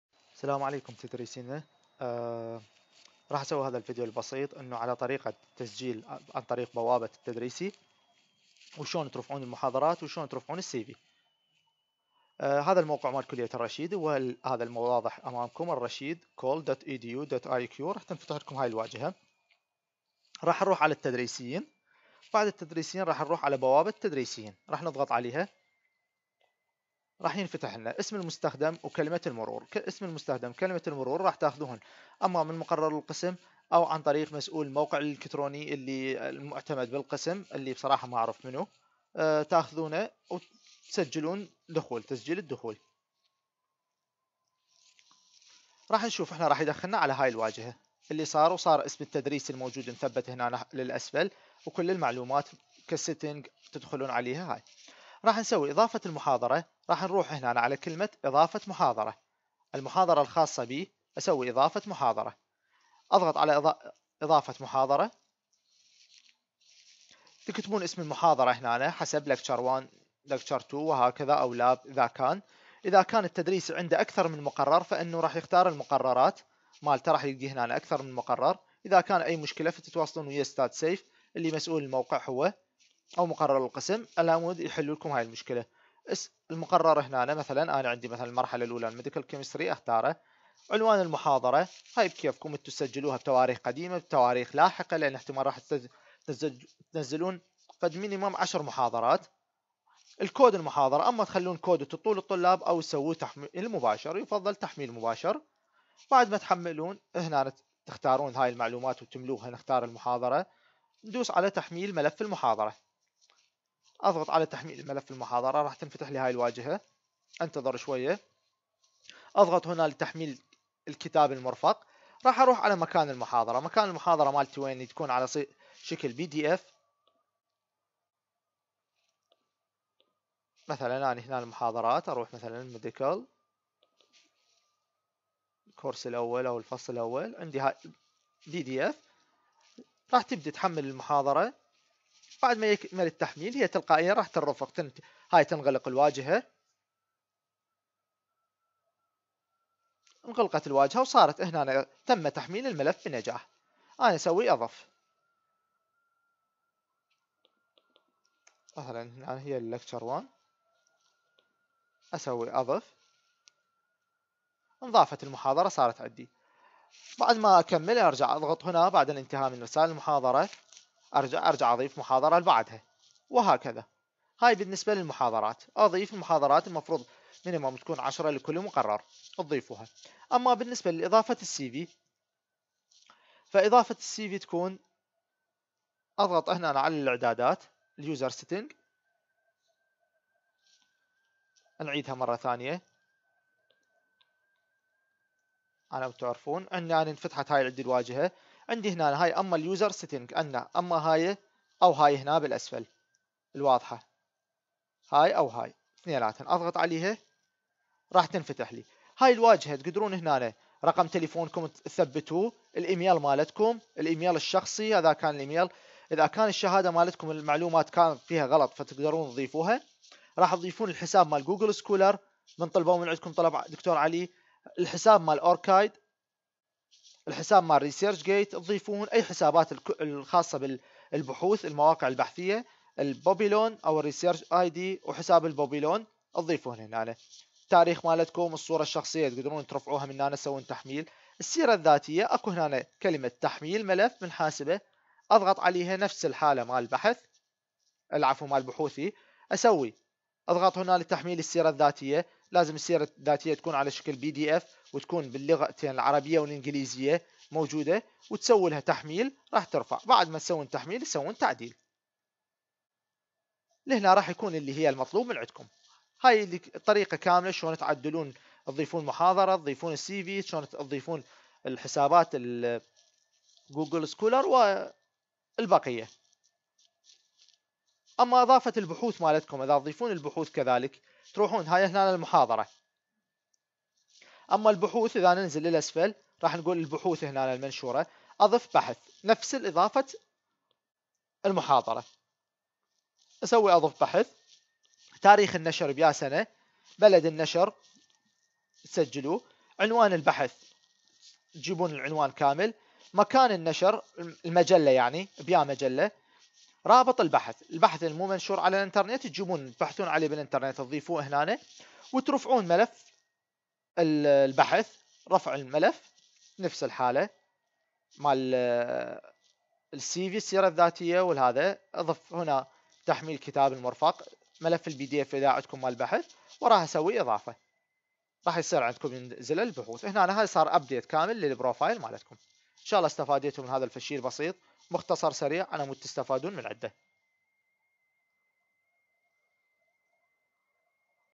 تحديث بروفايل حساب التدريسي في موقع كلية الرشيد
طريقة تحديث بروفايل حساب التدريسي في موقع كلية الرشيد الجامعة